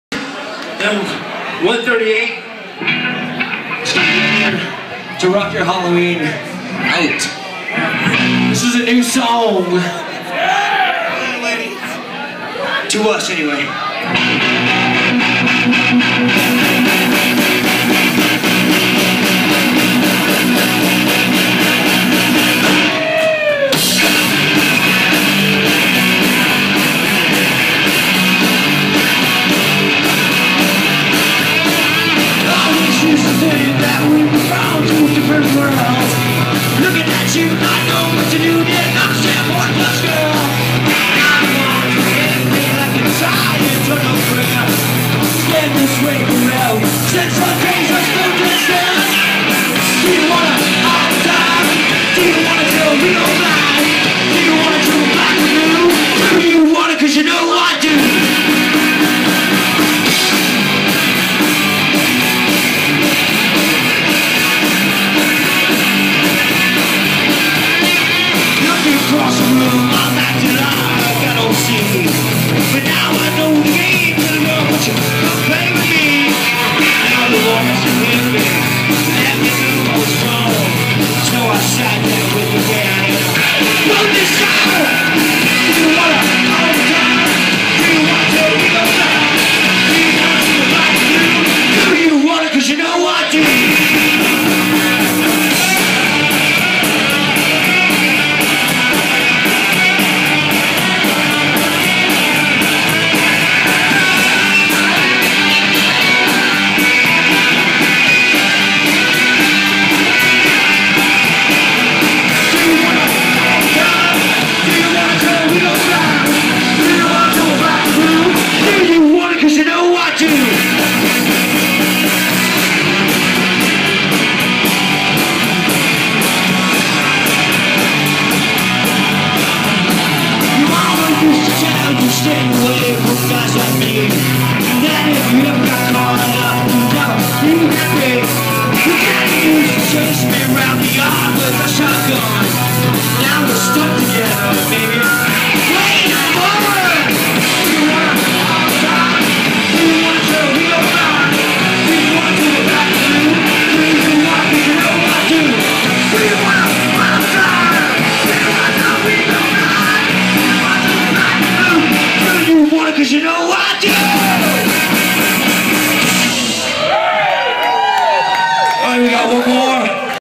BB Halloween
The Bottle Babies Live Halloween Night at The Belmont House of Smoke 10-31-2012